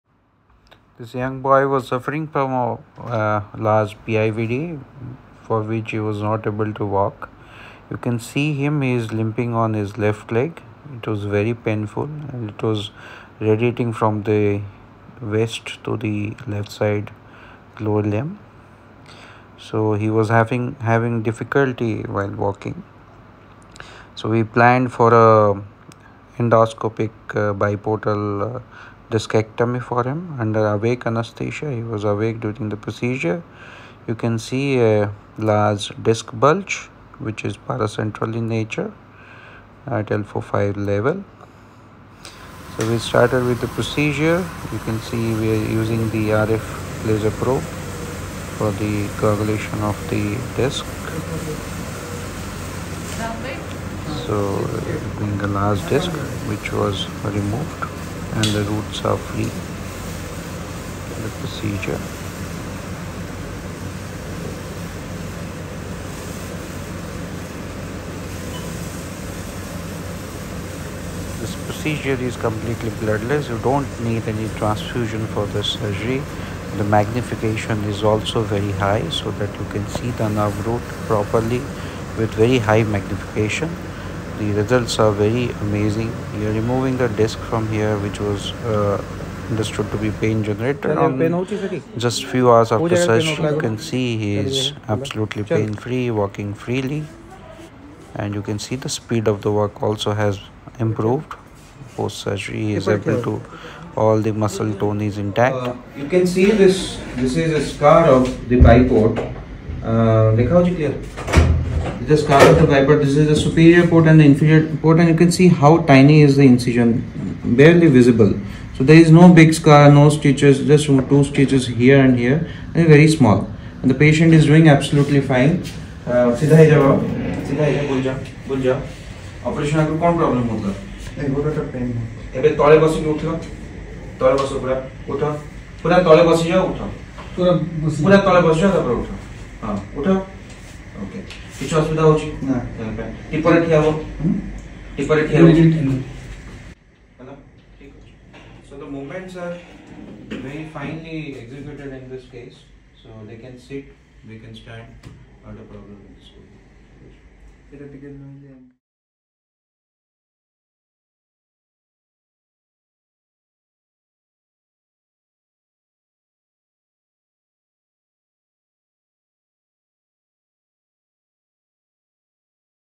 laser spine surgery/endoscopic biportal spine surgery
this video is about endoscopic laser spine surgery and Biportal endoscopic spine surgery.This is scarless  procedure.patient can be discharged on next day and can do all the normal activities from first day of surgery